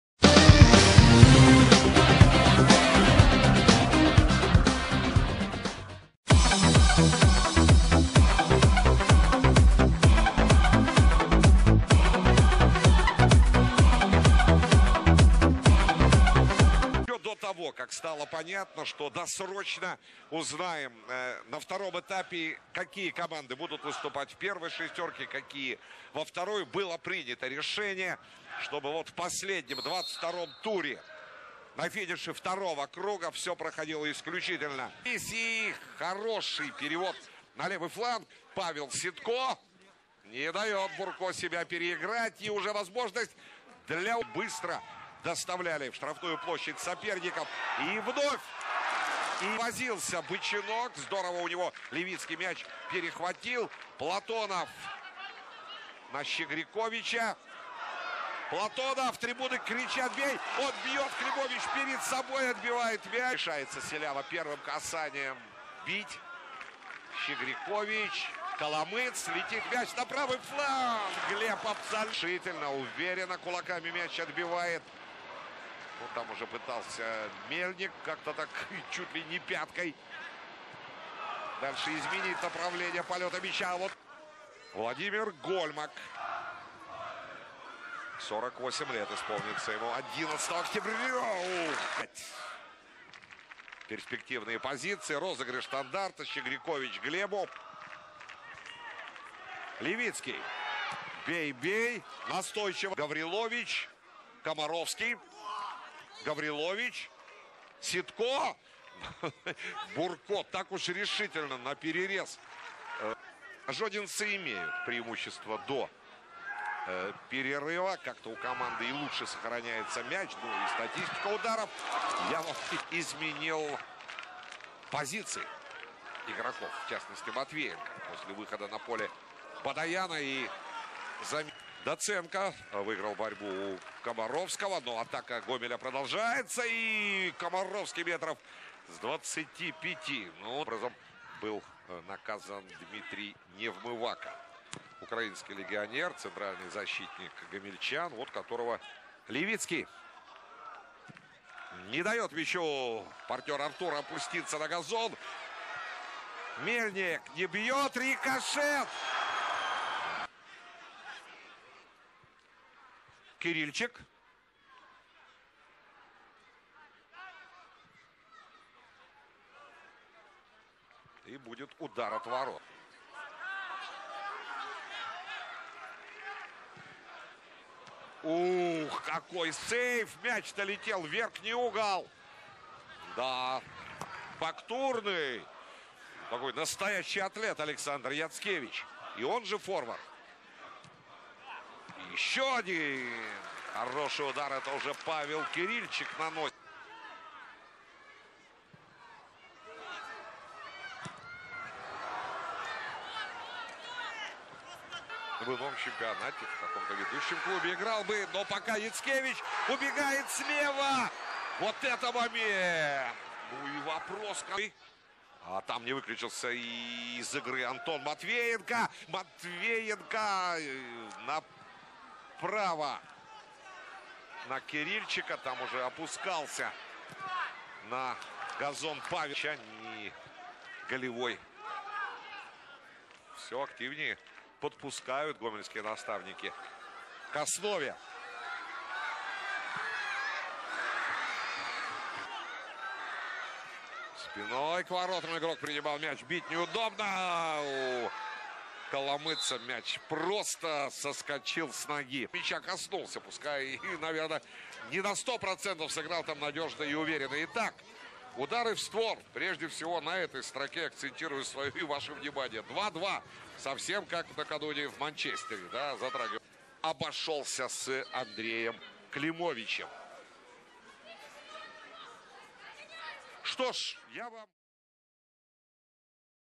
Торпедо-БелАЗ 0-0 Гомель. 31.08.2014
Видеообзор матча 22 тура чемпионата Беларуси по футболу Торпедо-БелАЗ 0-0 Гомель. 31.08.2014